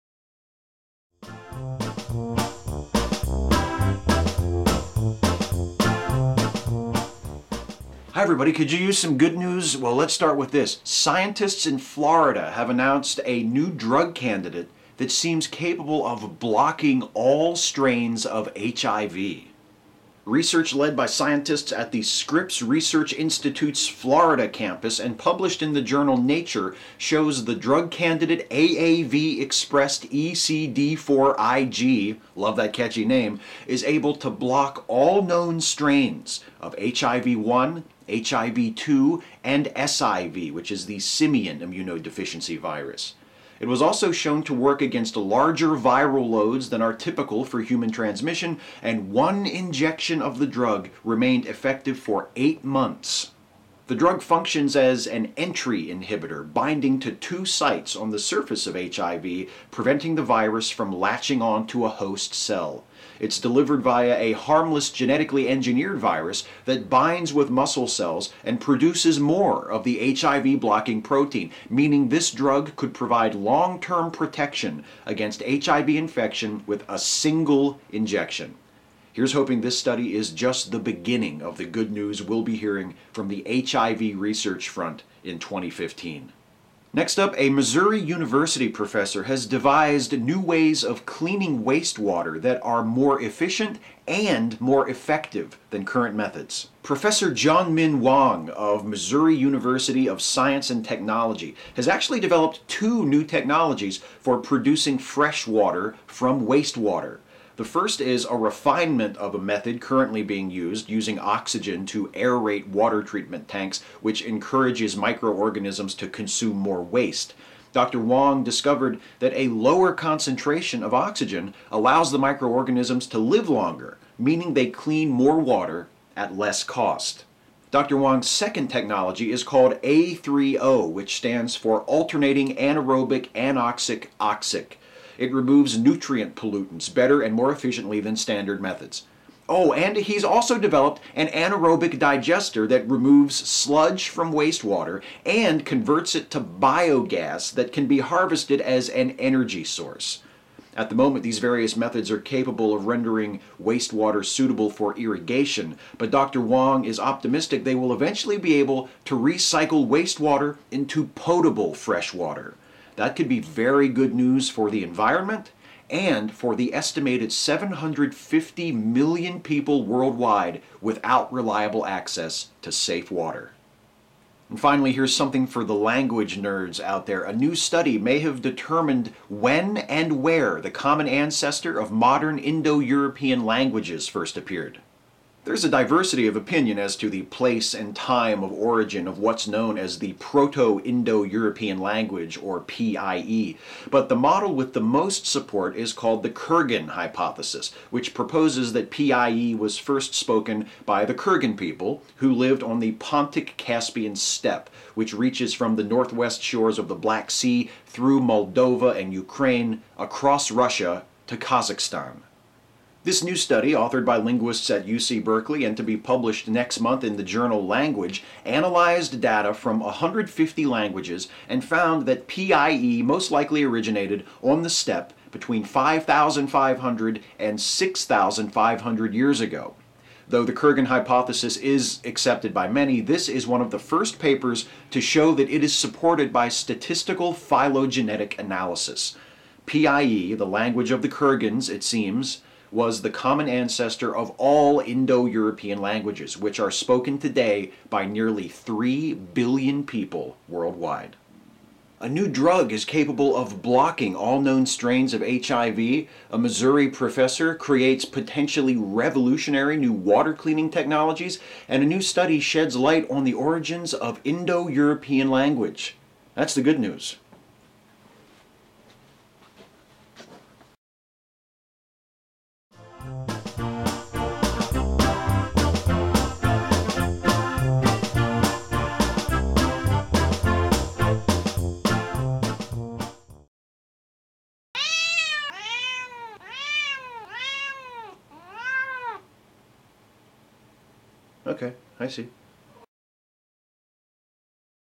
And Now the Good News #125: 2/24/2015
“Scripps Florida Scientists Announce Anti-HIV Agent So Powerful It Can Work in a Vaccine”

“Indo-European languages emerged roughly 6,500 years ago on Russian steppes, new research suggests”

Thanks!